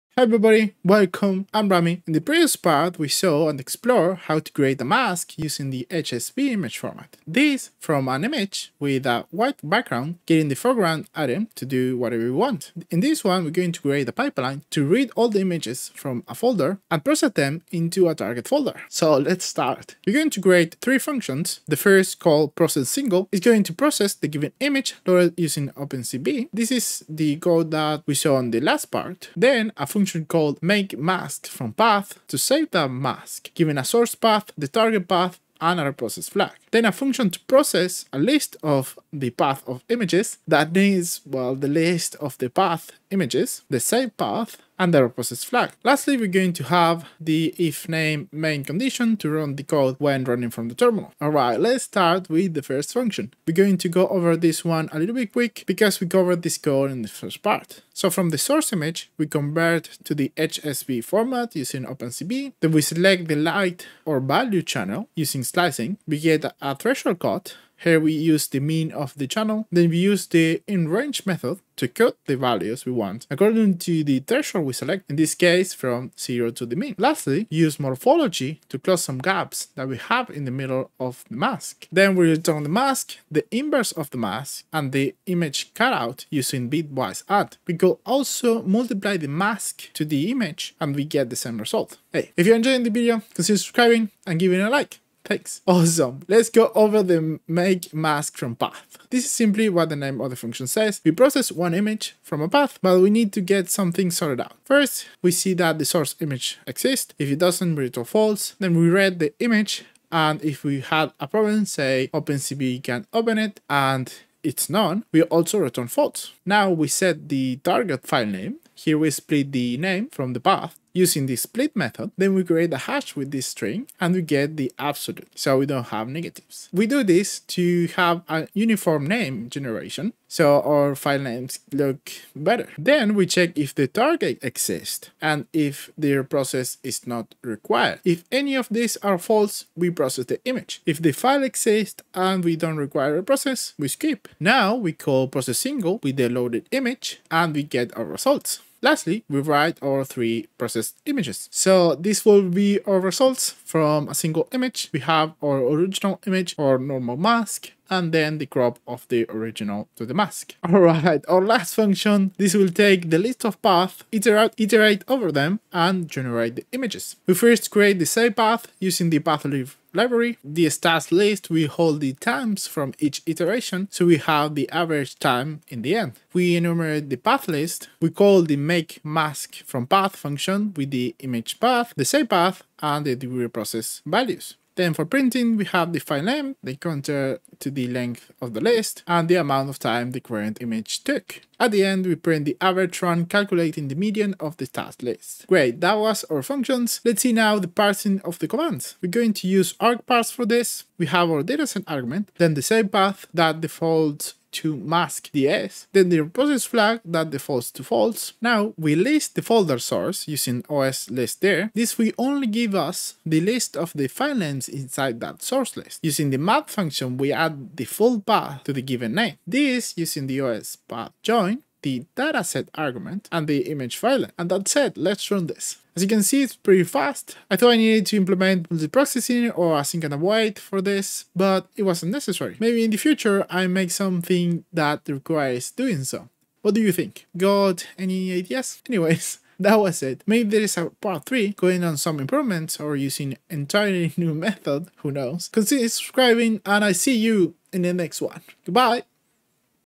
OpenCV Image processing Brightness mask #2 - Computer vision for Beginners
In this video, we use python and OpenCV to make a processing pipeline to process image mask using HSV luma mask!